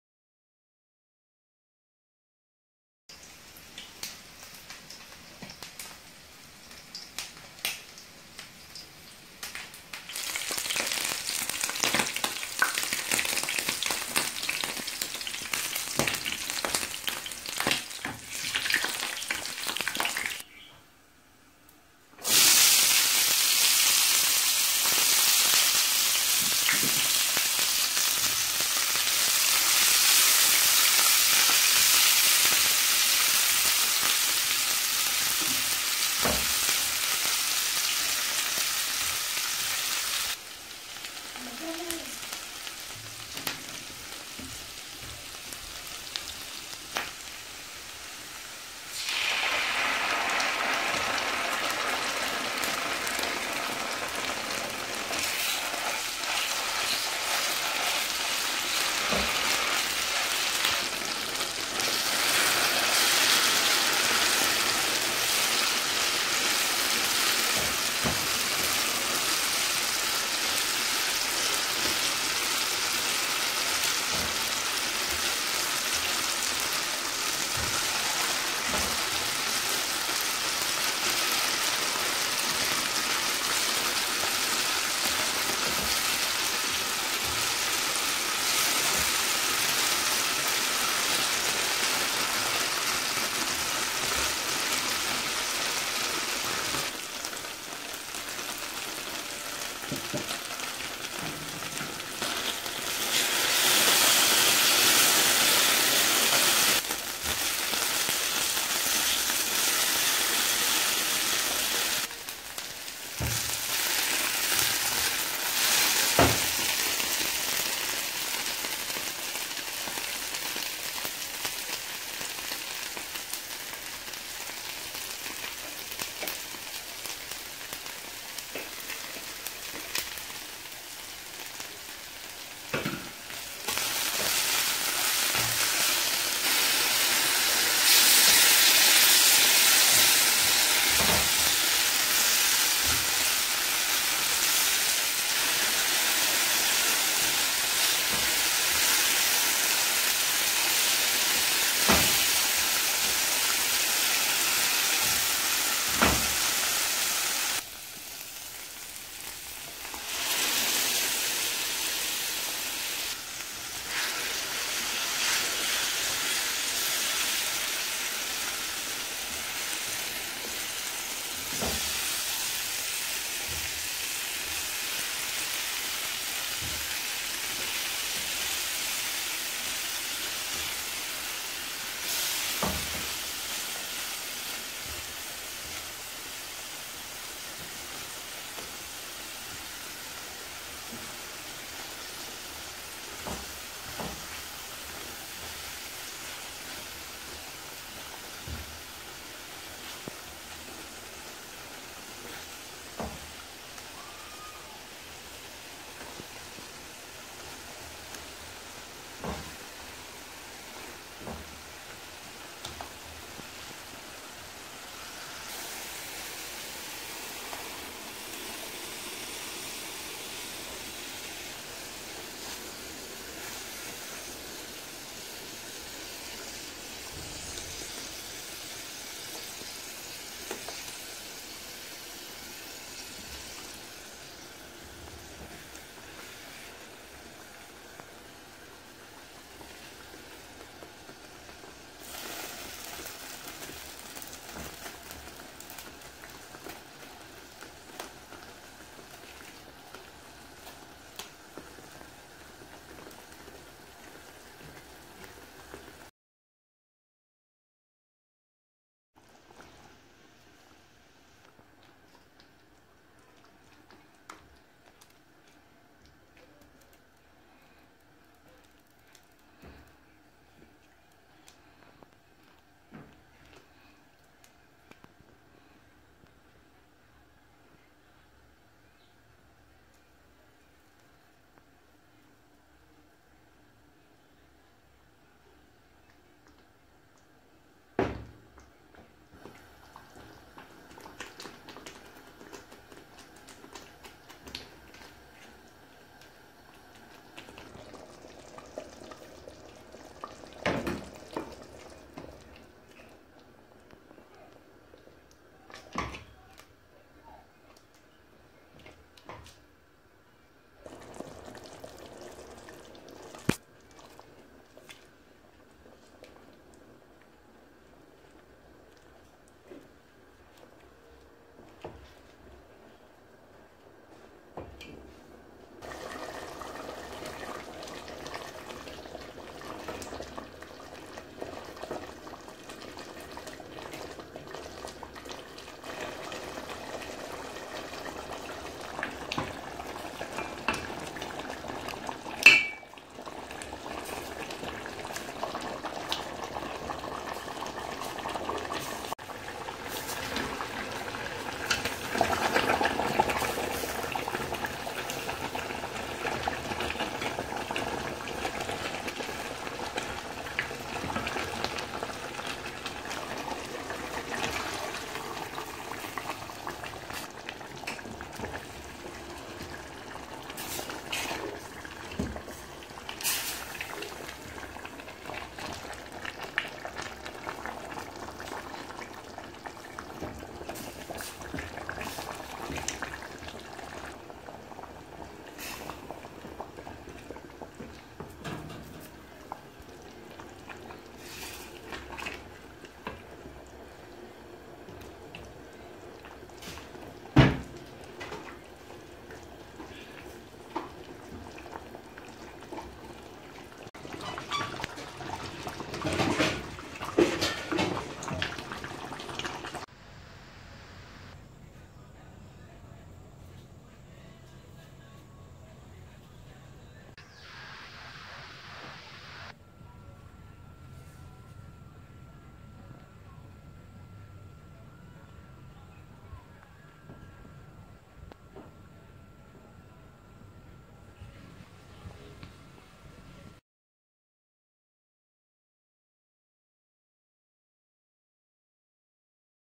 how to make the best chicken stew
ingredients
local chicken,
onions
tomatoes puree
tomatoe paste
salt
cumin, cardamon, mixed spices
and garam masala .
 oil
green paper.